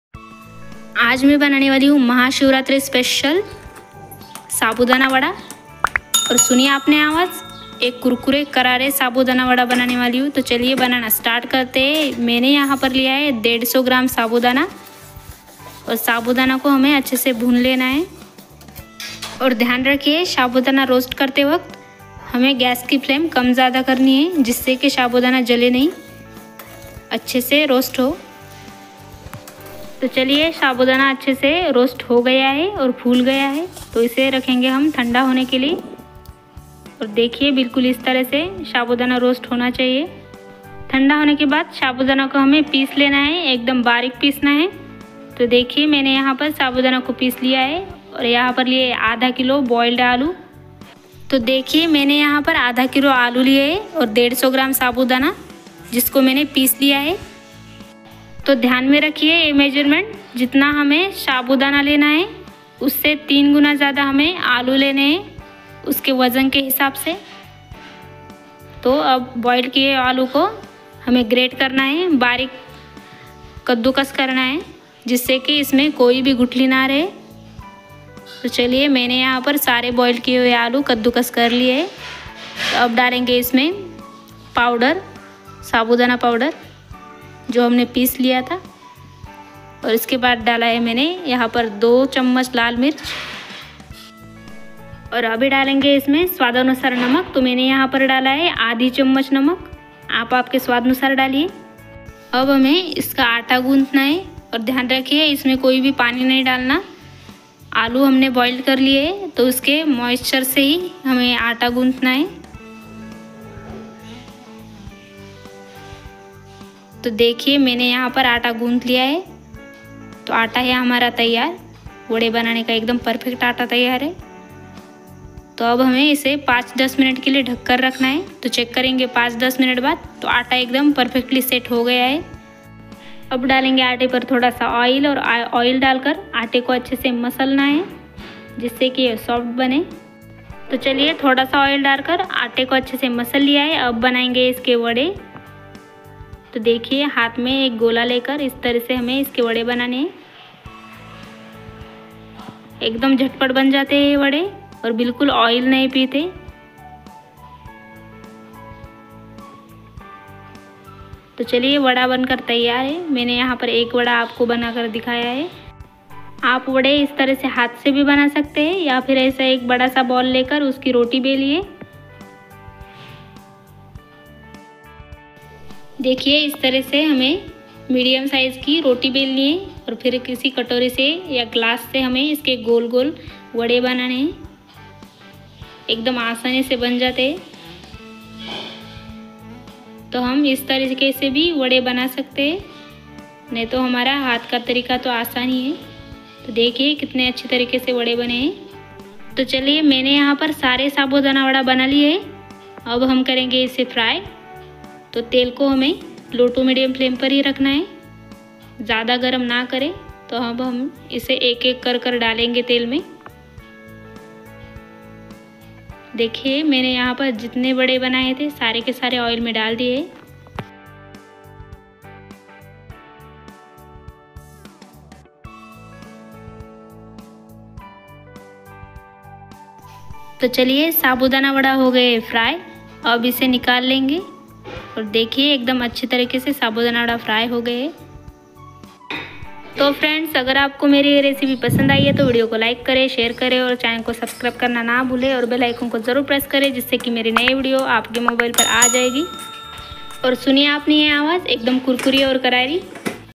बिना साबुदाना भिगोये बनाई ये झटपट बनने वाला साबुदाना वडा | sabudana vada recipe#vratrecipe
hello friends today I am making the video mahashivratri special sabudana vada recipe.

न साबूदाना भिगोना न आलू उबालना सबसे आसान टेस्टी क्रिस्पी साबूदाना का नया नाश्ता  Instant Sabudana Vada/Easy Vrat Recipes | महाशिवरात्रि व्रत रेसिपी | Instant Sabudana Vada Recipes